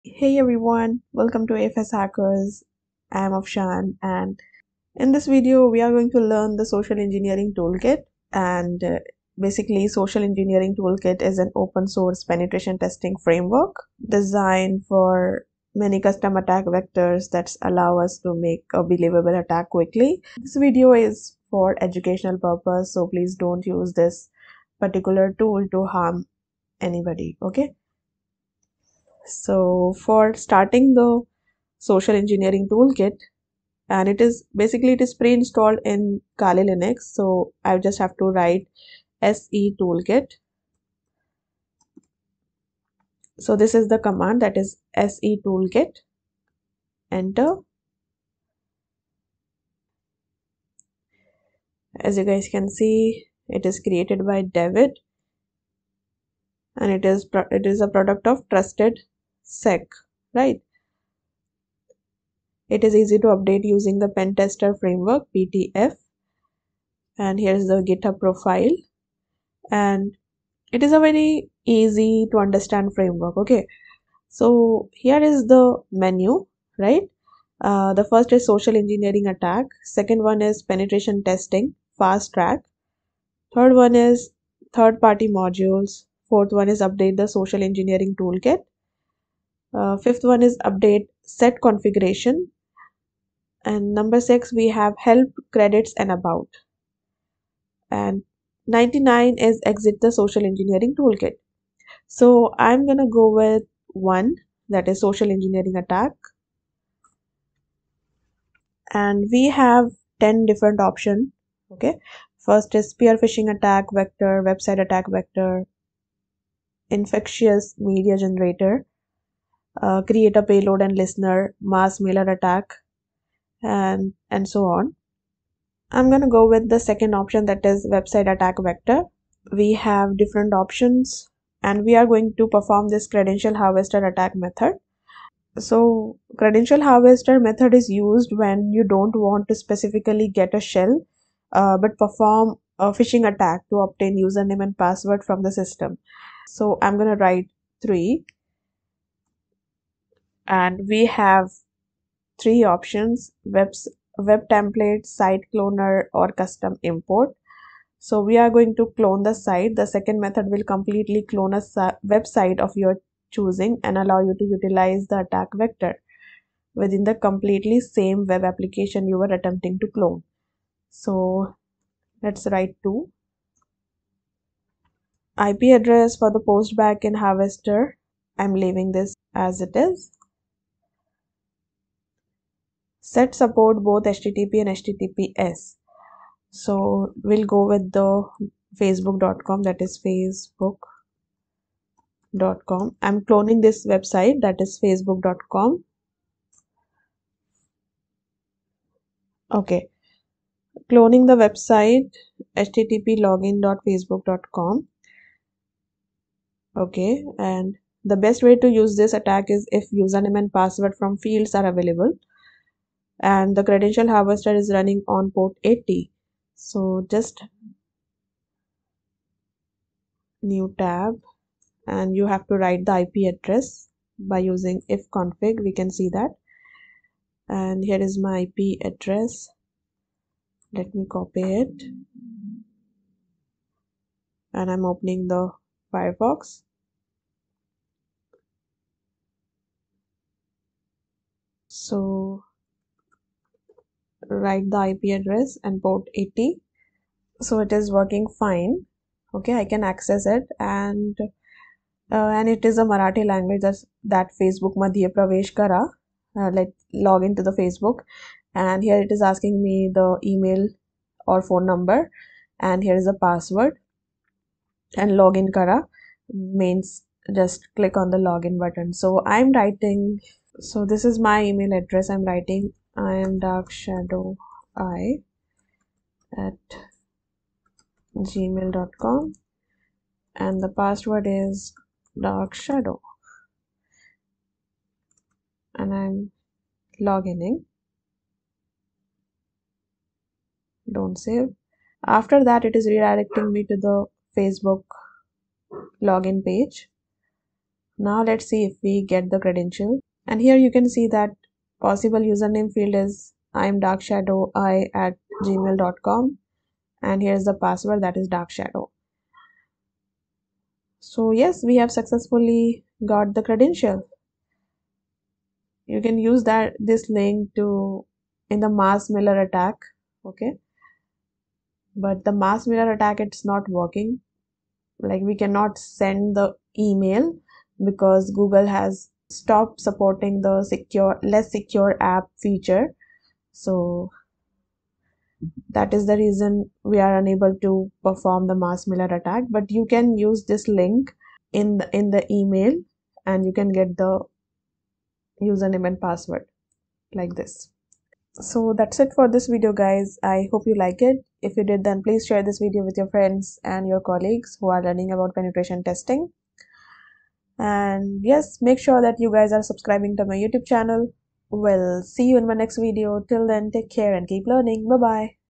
#5 SEToolKit tutorial | 2022 | Pentesting tool
The Social-Engineer Toolkit (SET) is specifically designed to perform advanced attacks against the human element.
SET was written by David Kennedy (ReL1K) and with a lot of help from the community, it has incorporated attacks never before seen in an exploitation toolset. The attacks built into the toolkit are designed to be targeted and focused attacks against a person or organization used during a penetration test.